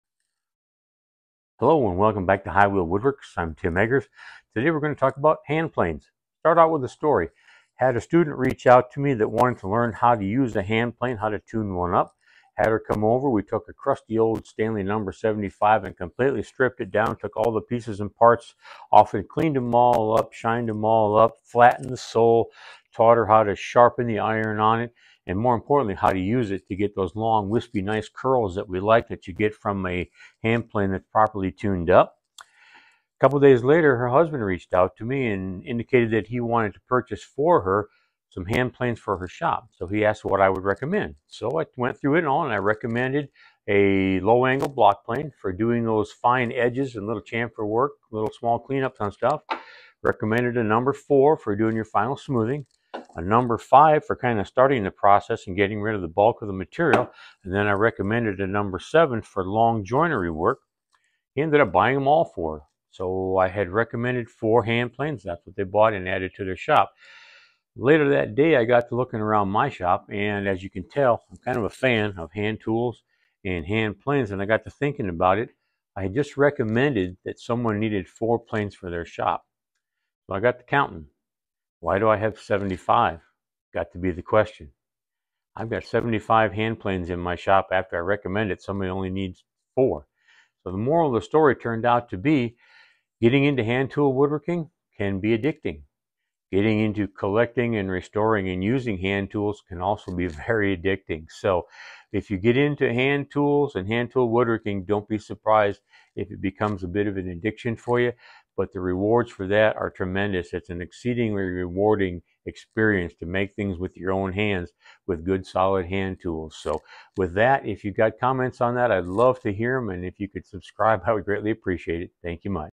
How many hand planes do you NEED in your shop?
My Hand plane recommendation to a new student:

1. Low Angle box plane for doing the fine edges and chamfer work
2. #4 for doing your final smoothing
3. #5 to get rid of the bulk
4. #7 for long joinery work

I have 75 hand planes- about 71 of them watch the other four in action! Have fun collecting.